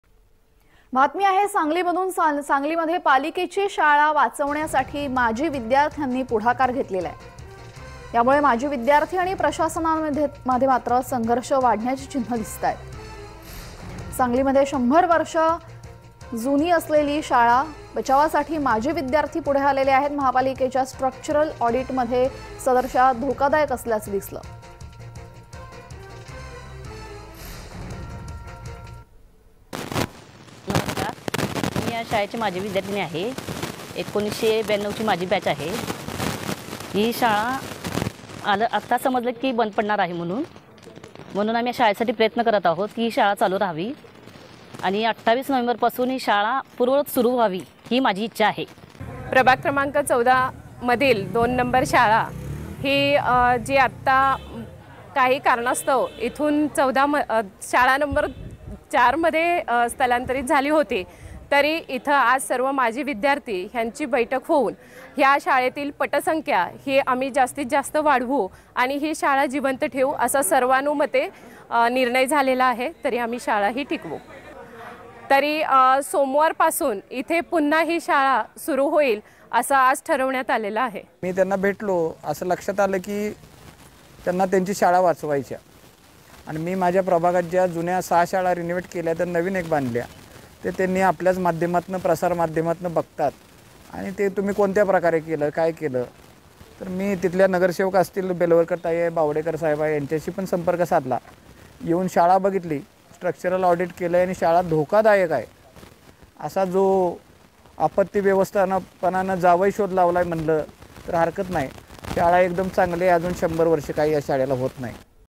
Sangli । पालिकेची शाळा बचावासाठी माजी विद्यार्थ्यांचा पुढाकार । Marathi News
पालिकेची शाळा बचावासाठी माजी विद्यार्थ्यांचा पुढाकार
माजी विद्यार्थी आणि प्रशासनात संघर्ष वाढण्याची शक्यता